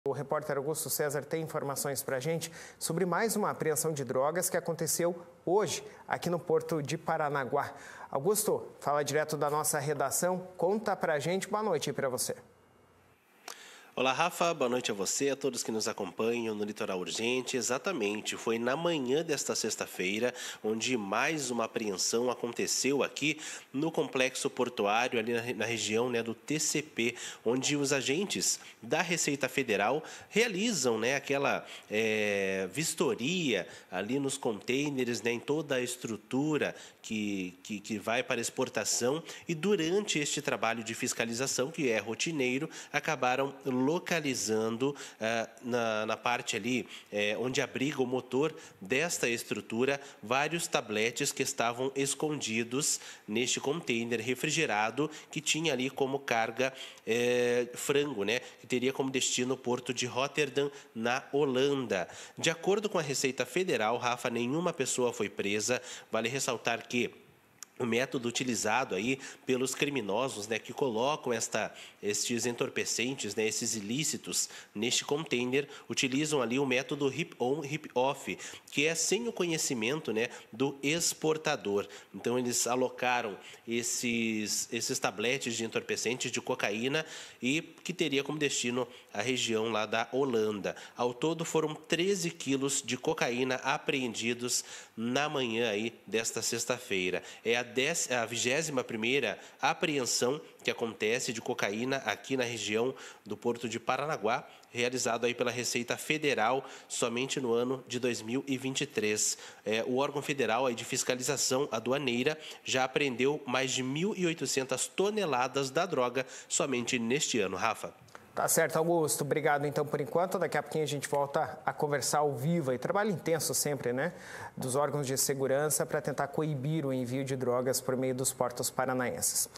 TRÁFICO INTERNACIONAL DE DROGAS
RECEITA FEDERAL APREENDE 13 KG DE COCAÍNA NO PORTO DE PARANAGUÁ; DROGA TINHA COMO DESTINO A HOLANDA